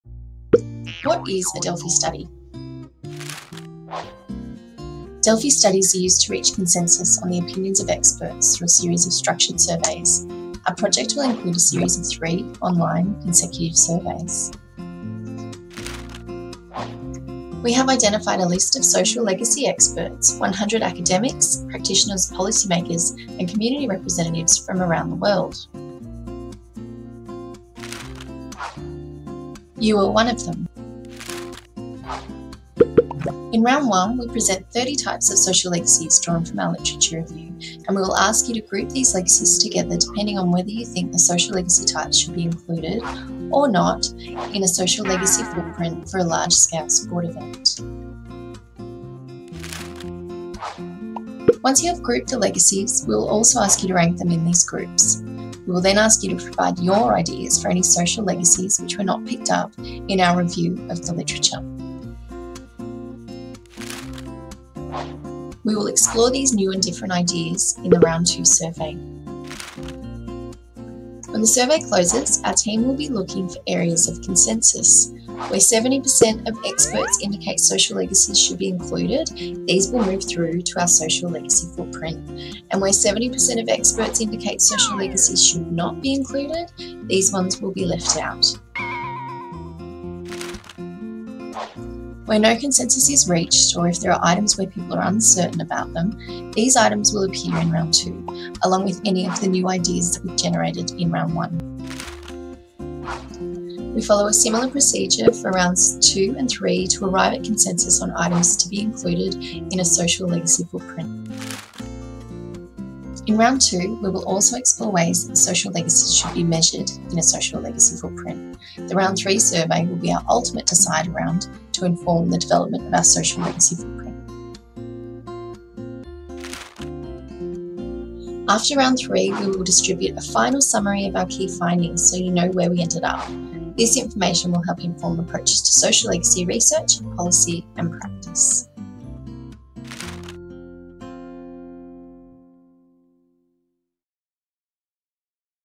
What is a Delphi study?
Script: What is a Delphi study?

Delphi studies are used to reach consensus on the opinions of experts through a series of structured surveys. Our project will include a series of three online consecutive surveys.

We have identified a list of social legacy experts - 100 academics, practitioners, policy makers and community representatives from around the world.

You are one of them.

In Round 1, we present 30 types of social legacies drawn from our literature review and we will ask you to group these legacies together depending on whether you think the social legacy types should be included, or not, in a social legacy footprint for a large-scale sport event.

Once you have grouped the legacies, we will also ask you to rank them in these groups. We will then ask you to provide YOUR ideas for any social legacies which were not picked up in our review of social legacies.

We will explore these new and different ideas in the Round 2 survey.

When the survey closes, our team will be looking for areas of consensus. Where 70% of experts indicate social legacies should be included, these will move through to our social legacy footprint, and where 70% of experts indicate social legacies should not be included, these will be left out.

Where no consensus is reached, or if there are items where people are uncertain about them, these items will appear in Round 2, along with any new ideas generated.

We follow a similar procedure for Rounds 2 and 3 to arrive at consensus on items to be included in a social legacy footprint.

In Round 2 we will also explore ways that the social legacies should be measured in a social legacy footprint. The Round 3 survey will be our ultimate decider round to inform the development of our social legacy footprint.

After Round 3, we will distribute a final summary of key findings so you know where we ended up. This information will help inform approaches to social legacy research, policy and practice.